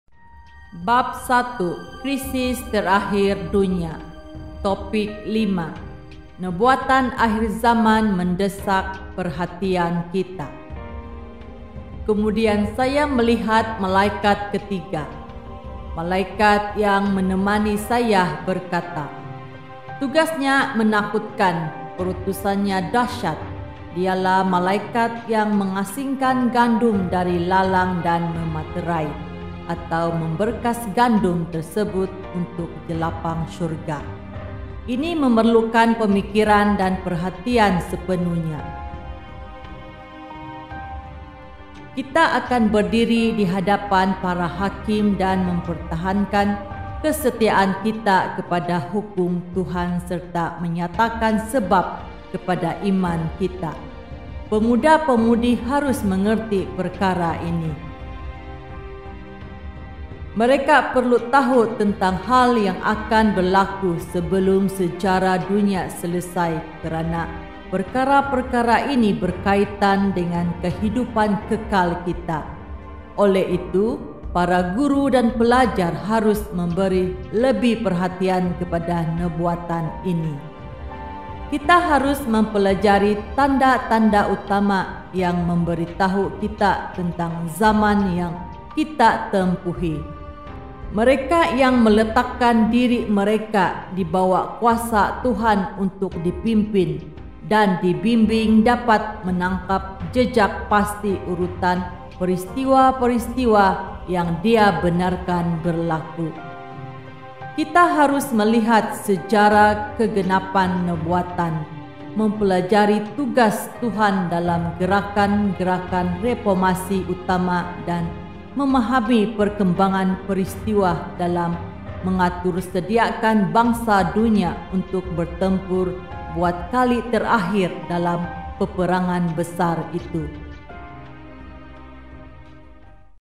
Peristiwa-peristiwa Akhir Zaman | Day 5# 05Feb2022
Bab 1 : Krisis Terakhir Dunia
Topik 5: Nubuatan Akhir Zaman Mendesak Perhatian Kita.
Kemudian saya melihat malaikat ketiga (Wah 14:9-11). Malaikat yang menemani saya berkata, “Tugasnya menakutkan. Perutusannya dahysat. Dialah malaikat yang mengasingkan gandum dari lalang dan mematerai, atau memberkas gandum tersebut untuk jelapang syurga. Ini memerlukan pemikiran dan perhatian sepenuhnya.” – EW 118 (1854). Kita akan berdiri di hadapan para hakim dan mempertahankan kesetiaan kita kepada hukum Tuhan serta menyatakan sebab kepada iman kita. Pemuda-pemudi harus mengerti perkara ini. Mereka perlu tahu tentang hal yang akan berlaku sebelum sejarah dunia selesai kerana perkara-perkara ini berkaitan dengan kehidupan kekal kita. Oleh itu, para guru dan pelajar harus memberi lebih perhatian kepada nubuatan ini. -6T 128 ,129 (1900). Kita harus mempelajari tanda-tanda utama yang memberitahu kita tentang zaman yang kita tempuhi. -4MR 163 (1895). Mereka yang meletakkan diri mereka di bawah kuasa Tuhan untuk dipimpin dan dibimbing dapat menanggap jejak pasti urutan peristiwa-peristiwa yang Dia benarkan berlaku. -7T 14 (1902).
Kita harus melihat sejarah kegenapan nubuatan,mempelajari tugas Tuhan dalam gerakan-gerakan reformasi utama dan memahami perkembangan peristiwa dalam mengatursediakan bangsa dunia untuk bertempur buat kali terakhir dalam peperangan besar itu. - 8T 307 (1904)